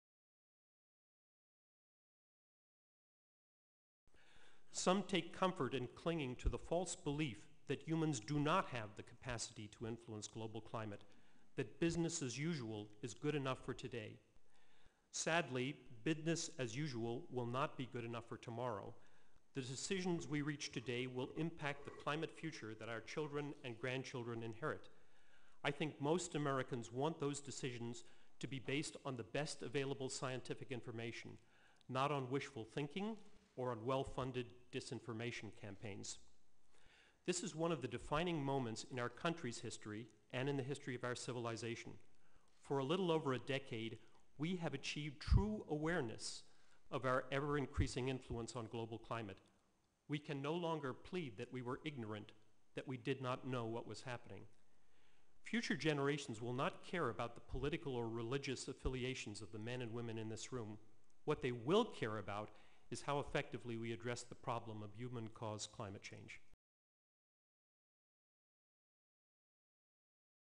Dr. Ben Santer's opening statement
Dr. Ben Santer, Research Scientist at the Lawrence Livermore National Laboratory , during the Select Committee on Energy Independence and Global Warming hearing, "Climate Science in the Political Arena" on May 20, 2010.